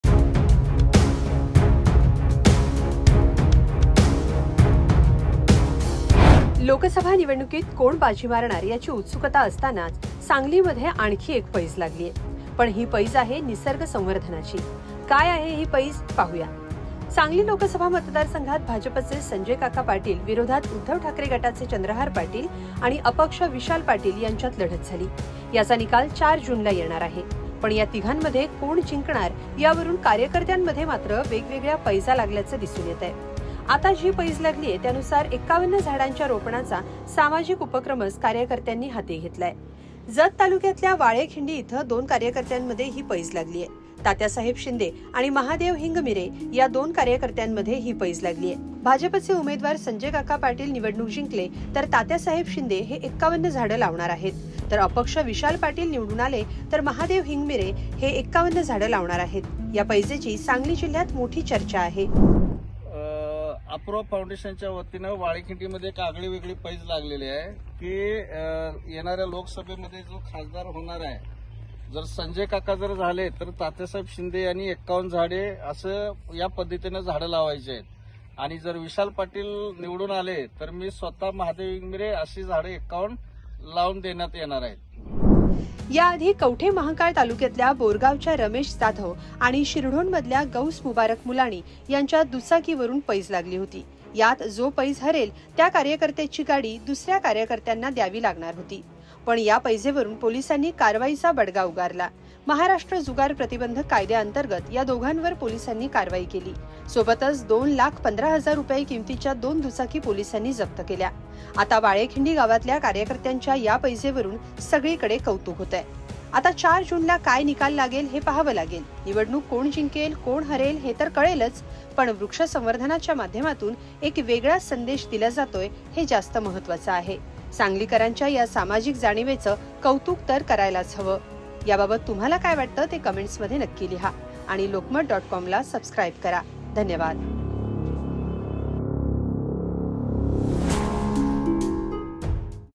निवडणूक निकालावरुन सांगली जिल्ह्यात लागली ‘ही’ पैज.. कार्यकर्त्यांचे का होतेय कौतुक? SP3
Bet of Plantation in Sangli | Lok Sabha Election निकालावरुन सांगली जिल्ह्यात लागली ‘ही’ पैज..कार्यकर्त्यांचे का होतेय कौतुक? Chandrahar Patil Sanjay Kaka Patyil Vidhal Patil